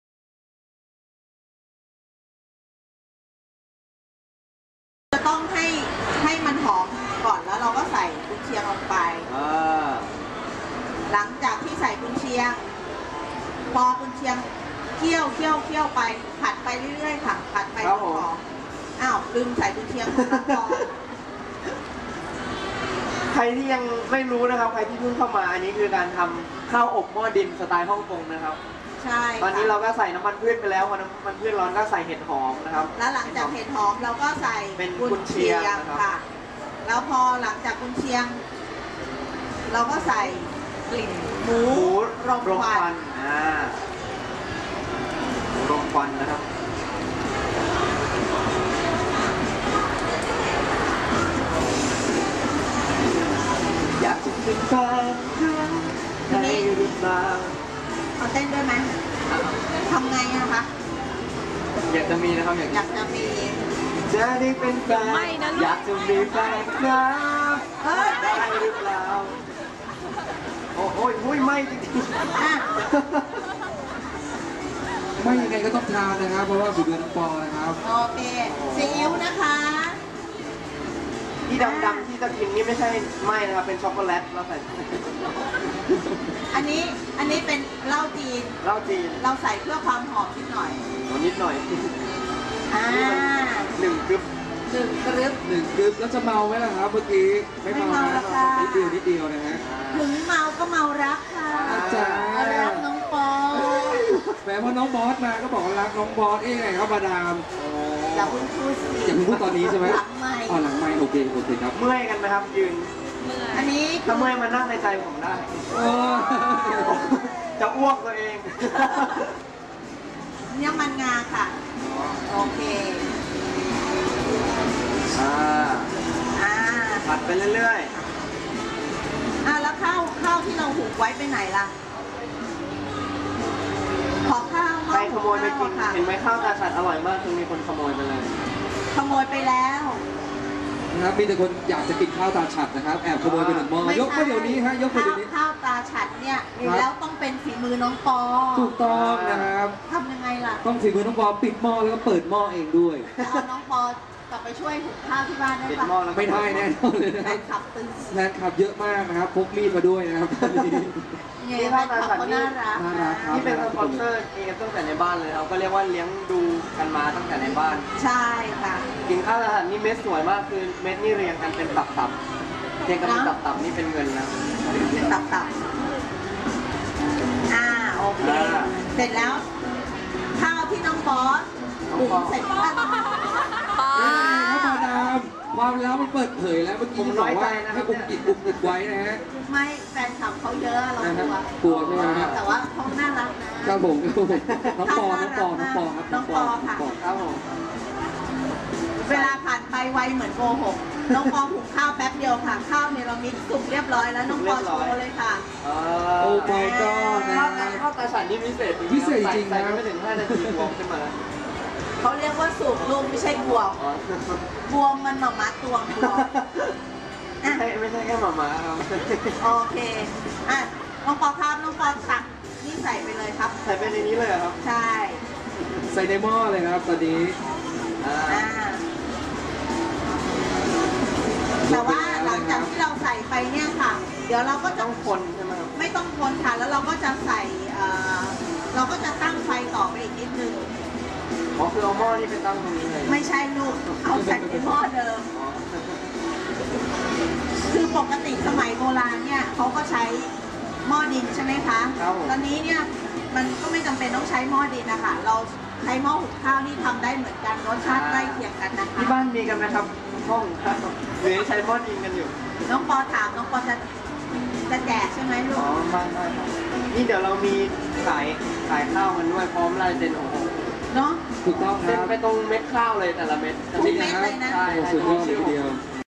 Por AF7 ทำข้าวอบหม้อดิน2 29May11
ปอ AF7 ทำข้าวอบหม้อดินสไตล์ฮ่องกง ตอน 2
บูธ ข้าวตราฉัตร
Thai FEX, Impact Challenger 2
29 May 2011